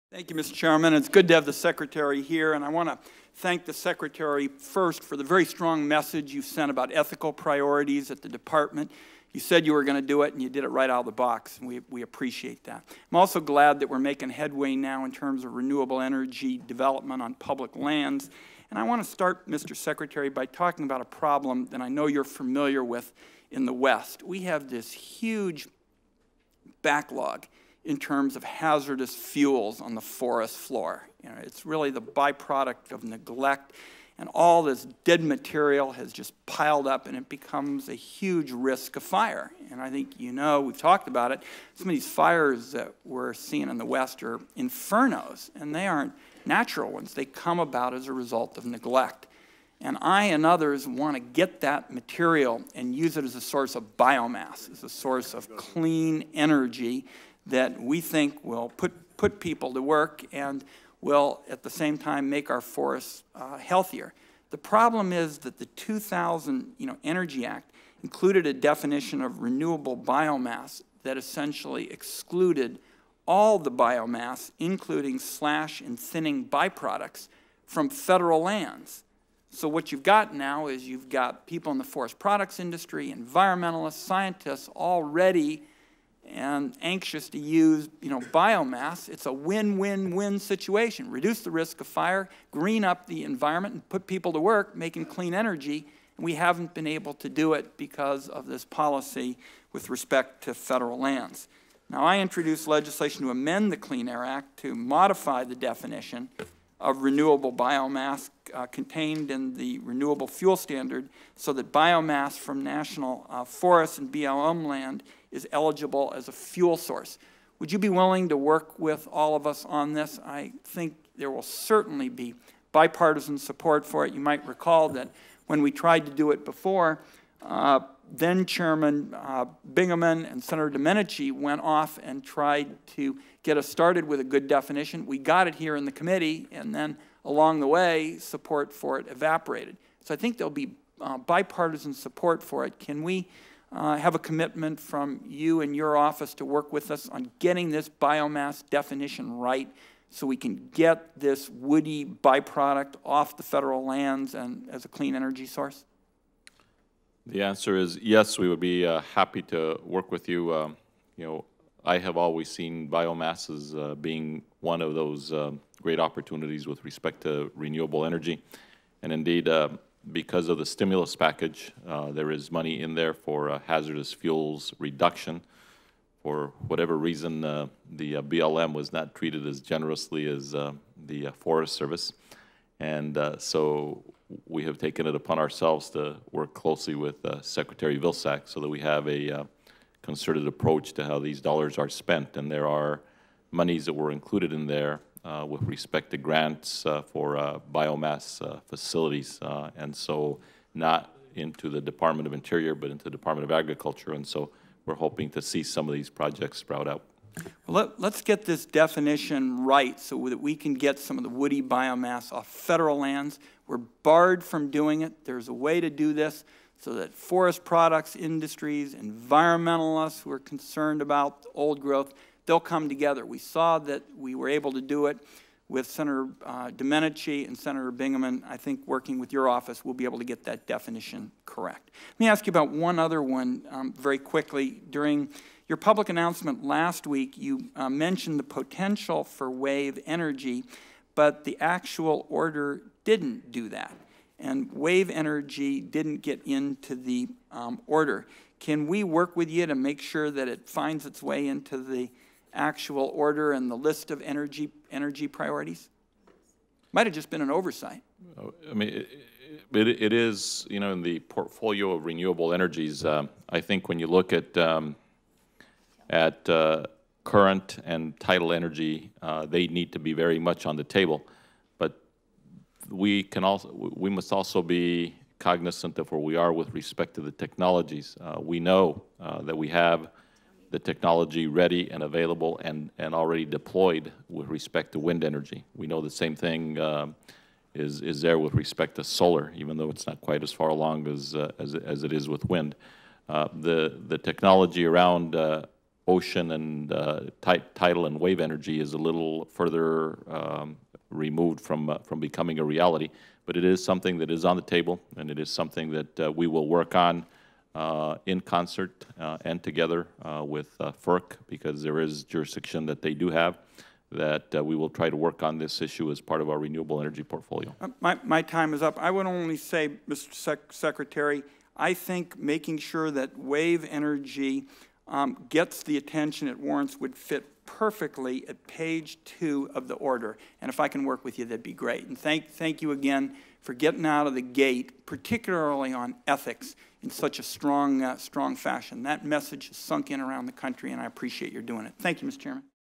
Renewable Energy Development on Public Lands
March 17, 2009: Senator Wyden questions Secretary of the Interior Ken Salazar during an Energy and Natural Resources Committee hearing on energy development.